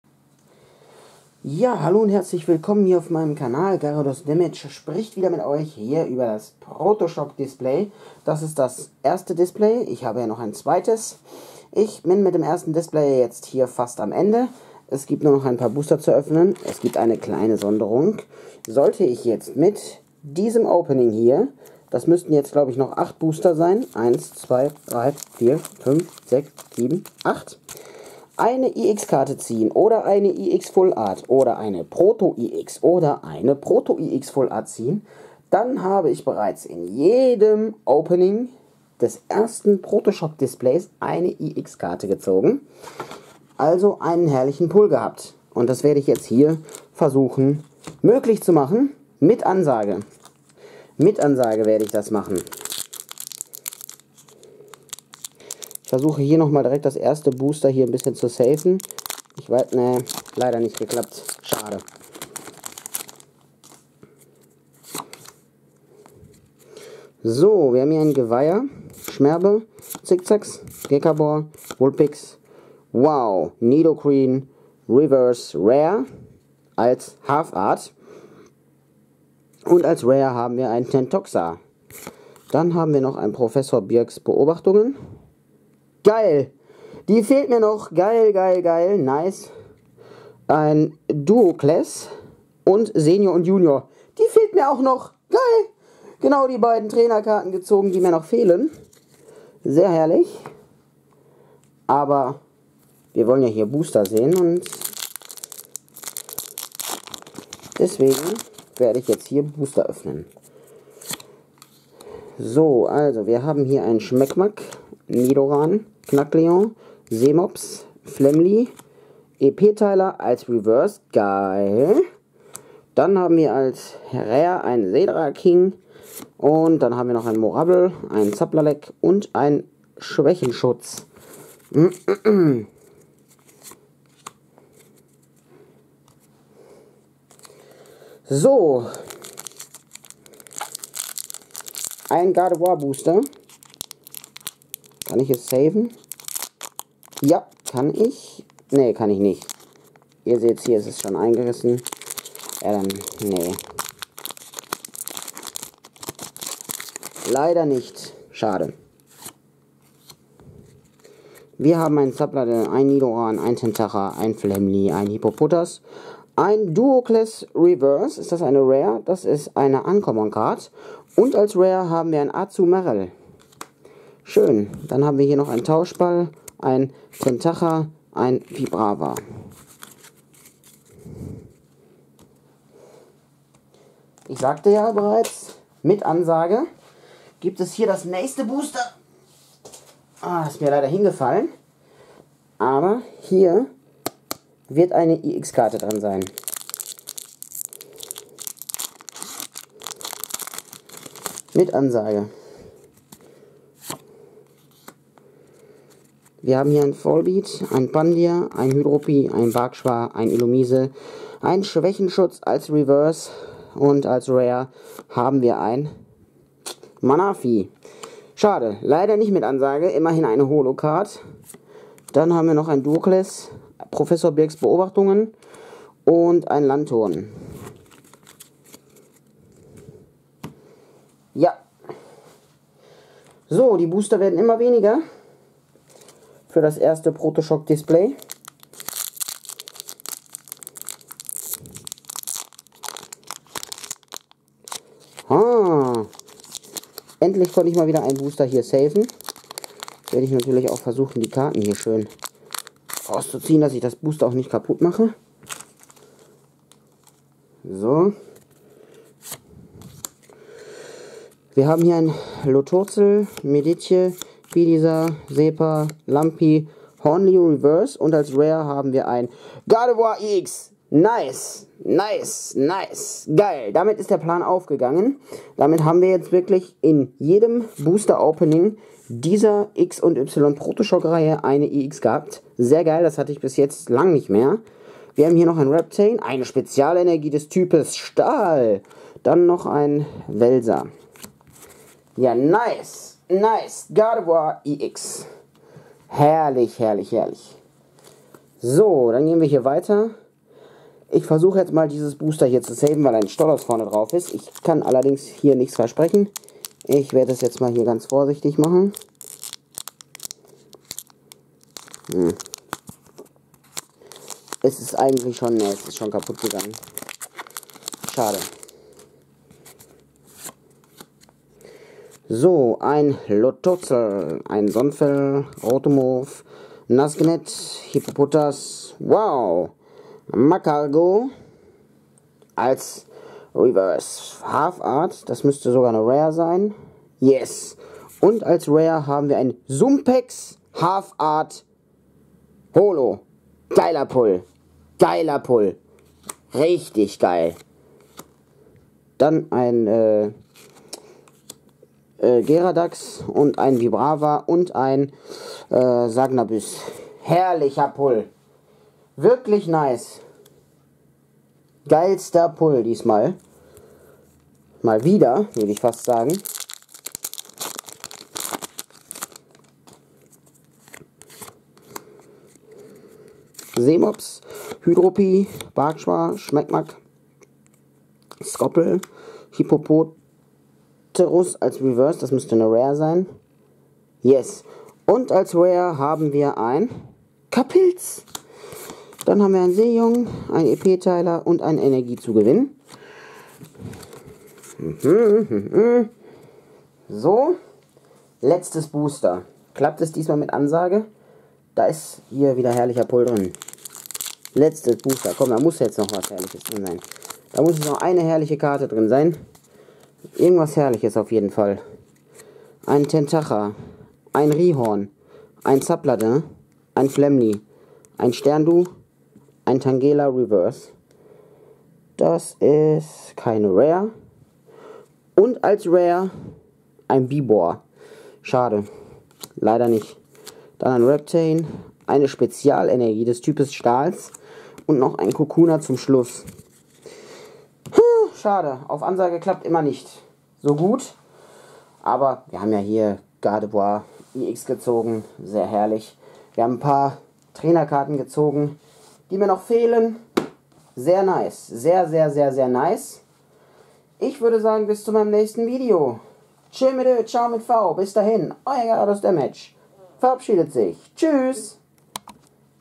Protoschock XY Teil 4
Es ist publik!

In jedem Teil meines Unboxings der Reihe Protoschock gab es einen EX Pull. Very Nice!